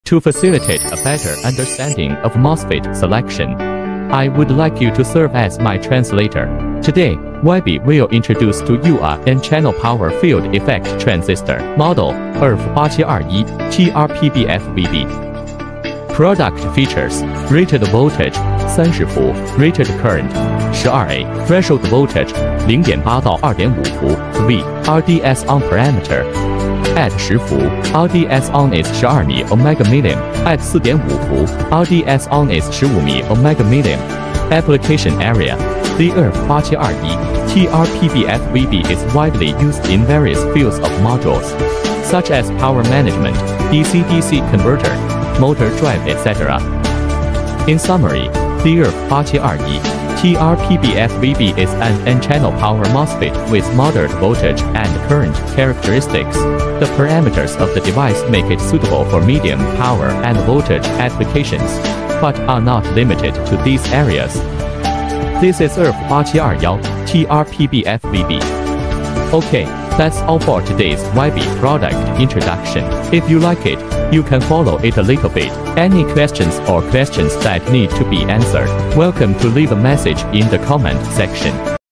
IRF8721TRPBF-VB, an SOP8 package N-channel MOS tube application field explanation
Model: IRF8721TRPBF-VB
  Silk screen: VBA1311
  Brand: VBsemi
  Parameters:
  - Channel type: N channel
  - Rated voltage: 30V
  - Maximum continuous current: 12A
  - Static turn-on resistance (RDS(ON)): 12mΩ @ 10V, 15mΩ @ 4.5V
  - Gate-source voltage (Vgs): 20V (±V)
  - Threshold voltage range (Vth): 0.8V to 2.5V
  - Package type: SOP8

  Application Brief:
  IRF8721TRPBF -VB is an N-channel power MOSFET with medium voltage and current characteristics suitable for a variety of electronic applications. The following are some possible application areas of the module:

  1. **Power module**: This MOSFET is suitable for low to medium power power modules such as power switches, DC-DC converters and inverters. It can be used as a switching device to achieve efficient power conversion.

  2. **Motor drive module**: IRF8721TRPBF-VB can be used for motor control modules, including DC motors, stepper motors and brushless DC motor drives. Its low on-resistance helps improve motor efficiency and performance.

  3. **Battery Protection Module**: In portable devices and battery-powered systems, this MOSFET can be used in battery protection circuits to prevent over-discharge and over-charge.

  4. **Signal switch module**: It can also be used for low to medium frequency signal switching, such as audio signals or low-speed digital signals. Its low on-resistance helps reduce signal distortion.

  5. **LED driver module**: In LED lighting systems, IRF8721TRPBF-VB can be used to drive LEDs to achieve brightness control and color adjustment.

  These are some examples of application area modules where the IRF8721TRPBF-VB N-channel MOSFET may be used. The parameters of this device make it suitable for, but not limited to, medium power and voltage applications. In actual applications, it is important to adhere to the electrical specifications and operating conditions in the data sheet to ensure optimal performance and reliability.